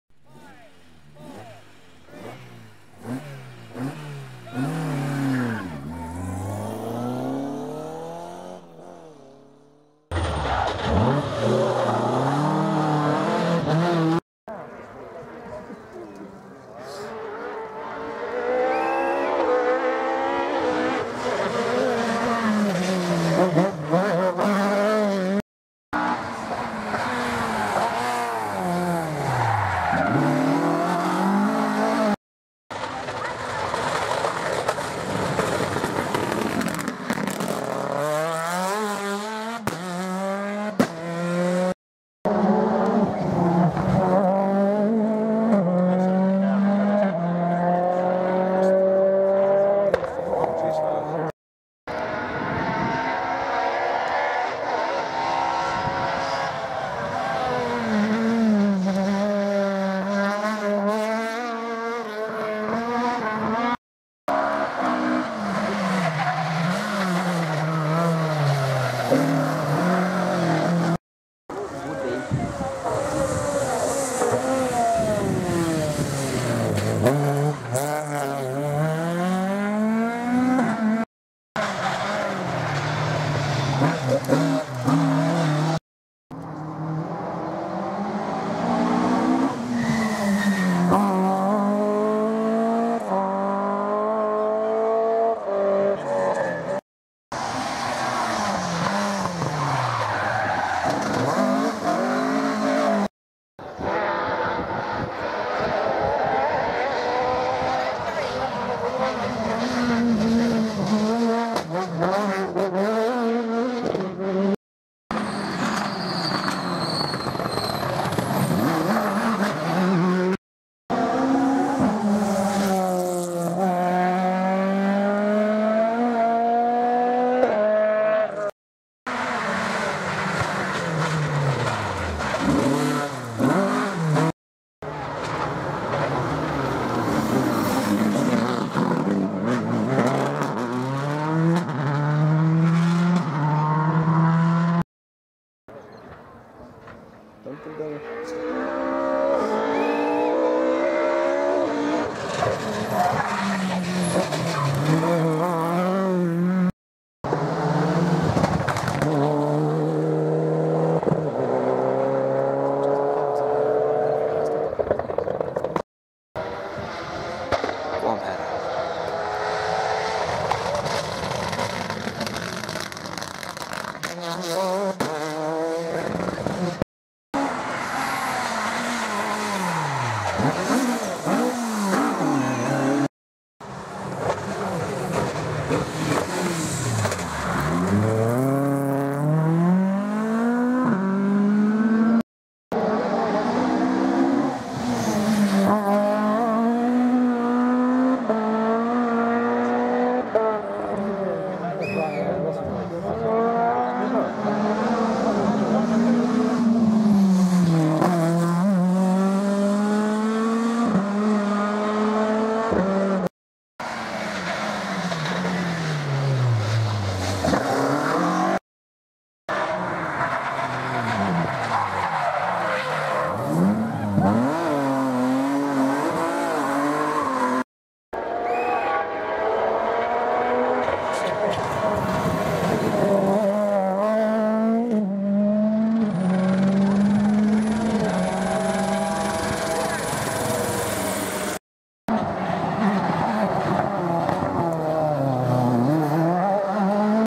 Monaghan Stages Rally 2014.
A video of from the 2014 Hillgrove Hotel Monaghan Stages Rally. We were on Stages 1, 3, 5, 6 and 9. We only had one camera on the rally, so we tried to capture as much footage as we could.